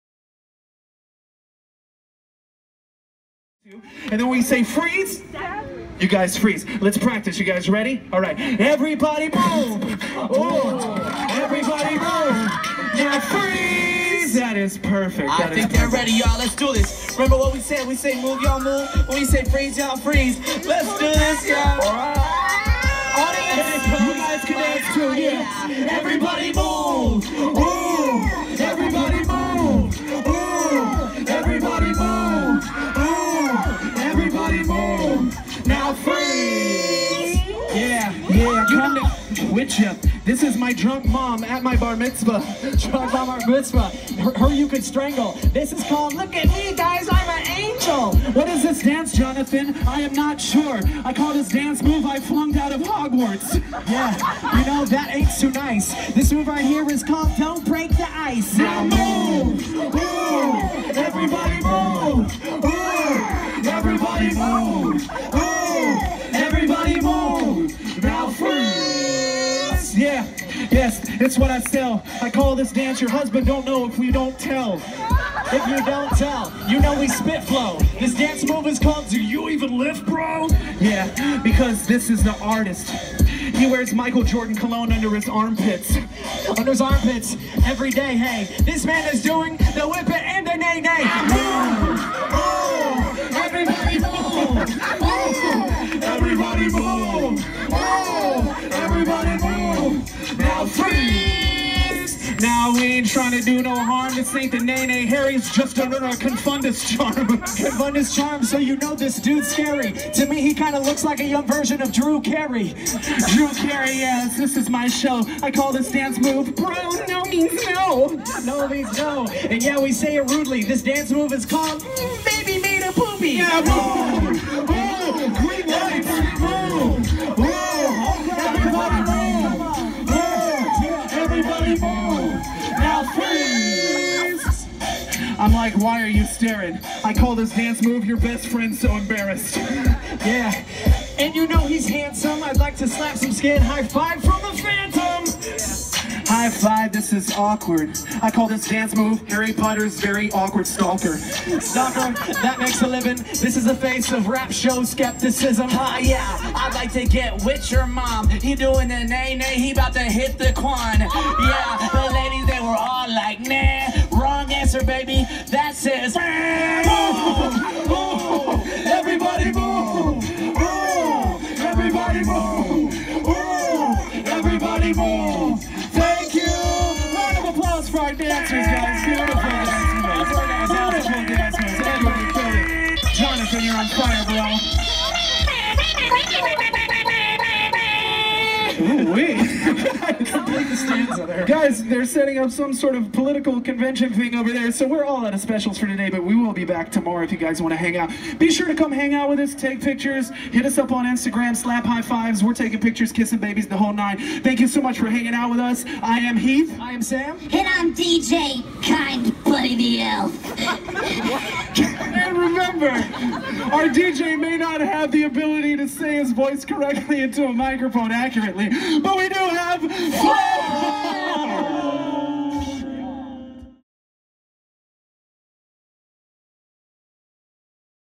Straight Outta Food Truck at Universal Studios Florida
Video of the finale for Straight Outta Food Truck at Universal Studios Florida. If you haven't had the chance to see this show yet, you definitely should. It's very entertaining and funny!